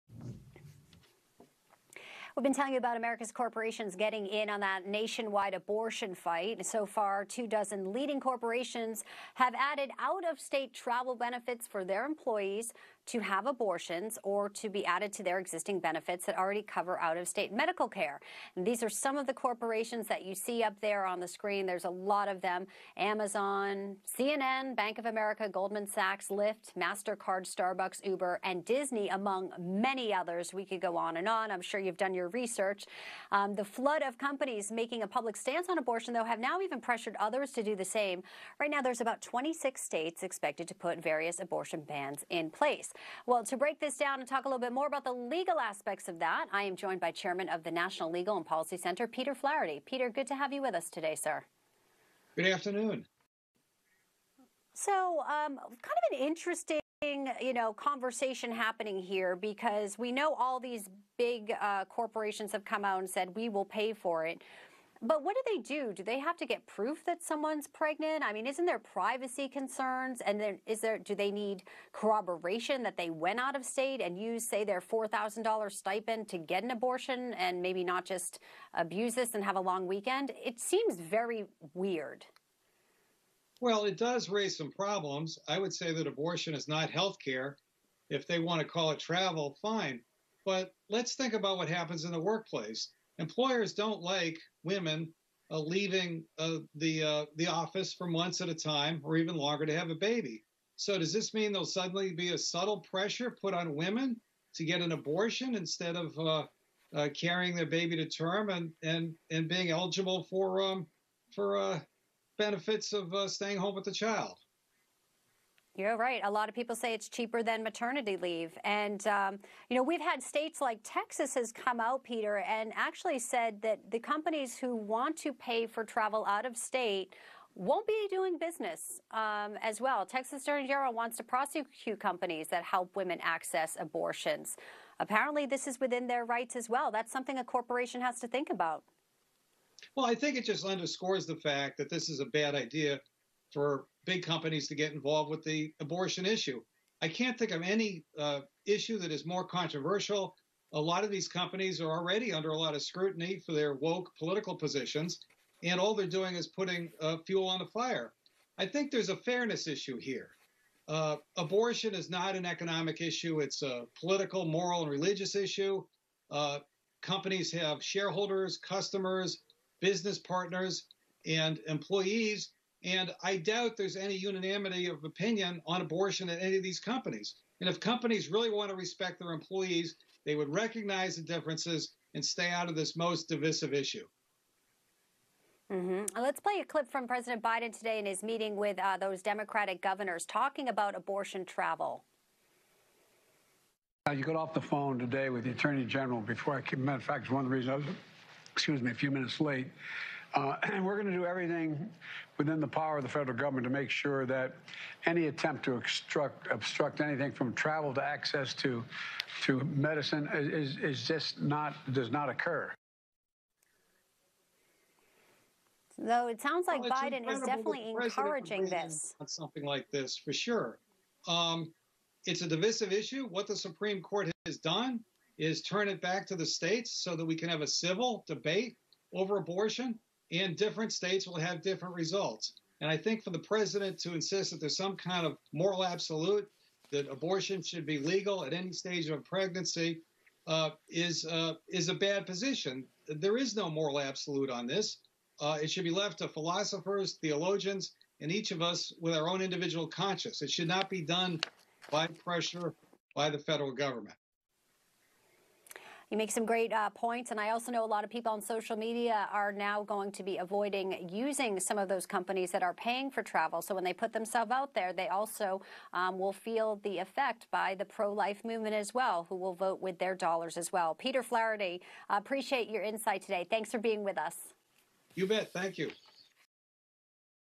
Corporate America Should Stay Out of Abortion Controversy
7/1/22- Peter Flaherty, Chairman of the National Legal and Policy Center, is interviewed on Newsmax TV.